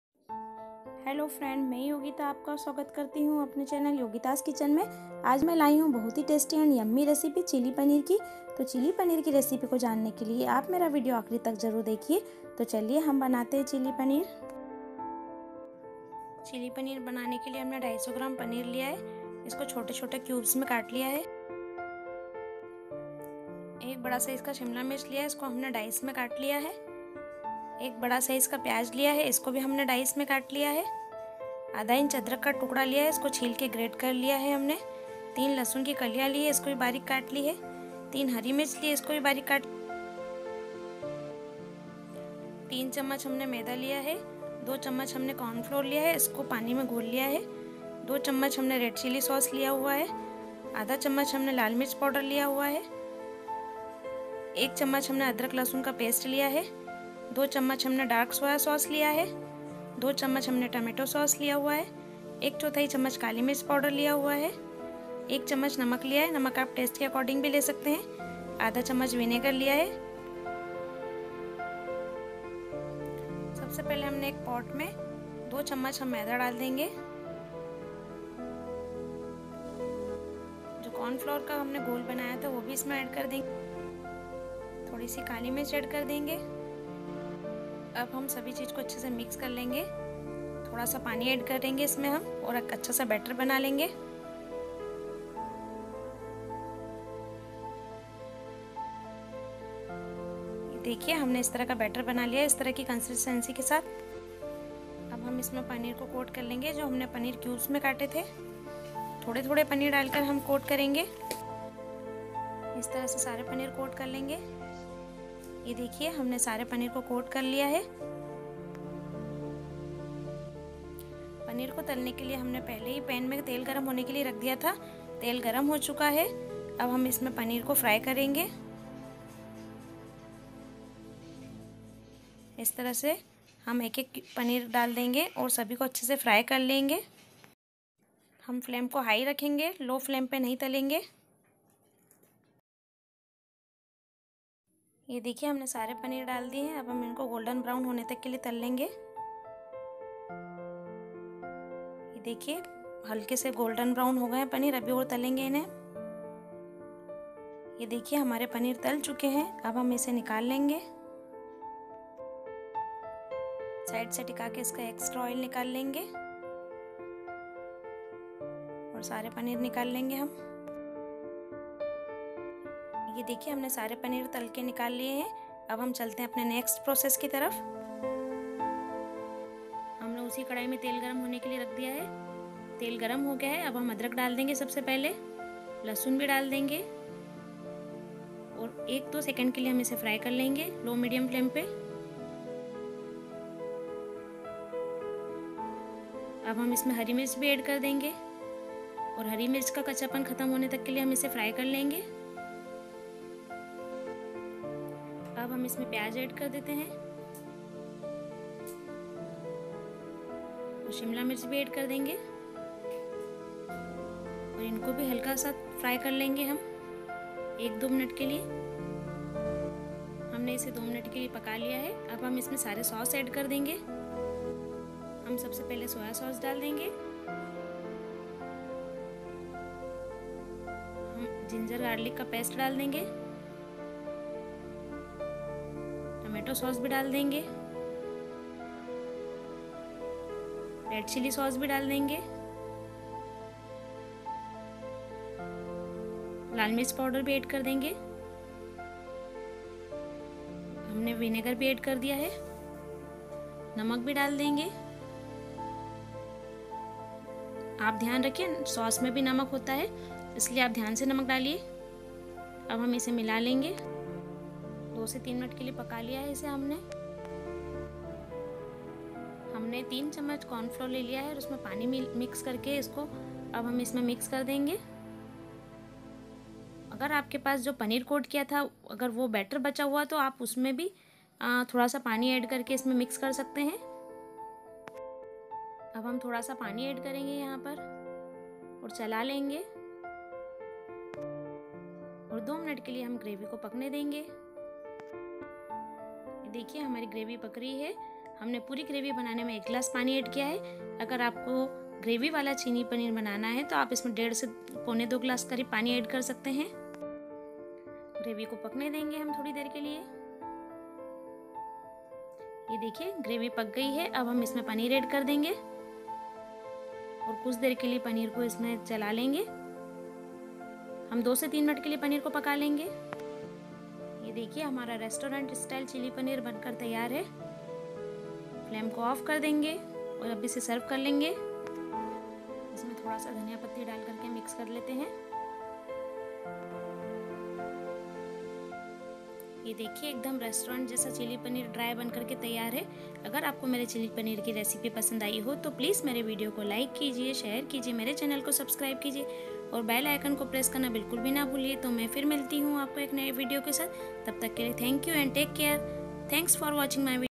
चिली पनीर/जब बनाएंगे ऐसा चिली पनीर तो सब तारिफ करेंगे by yogeeta chouksey
Hello friends,
               Today I am sharing my new recipe tasty and healthy chilli paneer. Do you try at your home and give your feedback yogeeta's kitchen

Chilli paneer
Ingredients:--

250 gr. Paneer
1 big size capcicum
1 big size Onion
1/2 Inch ginger
3 pice garlic
3 green chilli
3 tsp gram flour
3 tsp cornflour
2 tsp red chilli souce
1/2 tsp red chilli
1 tsp Ginger garlic past
2 tbs soya souce
2 tbs tomato souce
1/4 tsp black paper
1 tsp salt (according to test)
1/2 tsp vinegar
Some oil